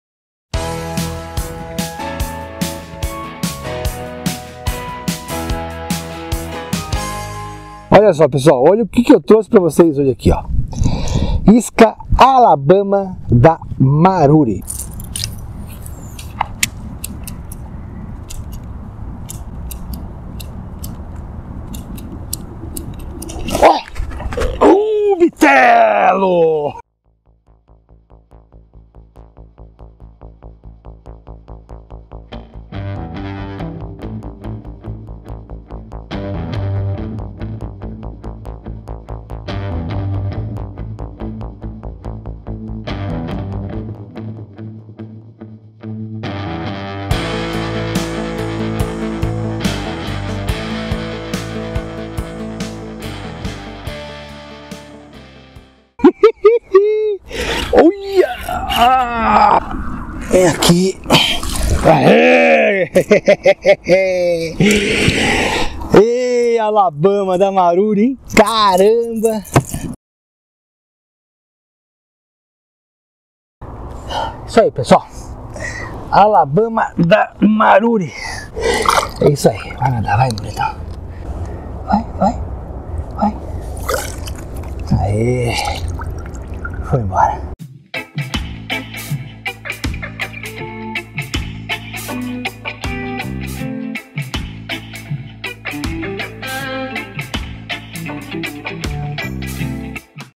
A ISCA QUE SALVA PESCARIAS!!!!!!!
Hoje trago para você um vídeo demonstrativo da eficiência da isca Alabama da Maruri.
Sem dúvida nenhuma uma isca expetacular e que não pode faltar em sua caixa de pesca.
Abraços
P.G

Segue abaixo os links dos materiais utilizados nesta pescaria, só clicar para ver mais e poder também adquirir.

▬▬▬▬▬▬ Contatos profissionais  ▬▬▬▬▬▬▬
- Parcerias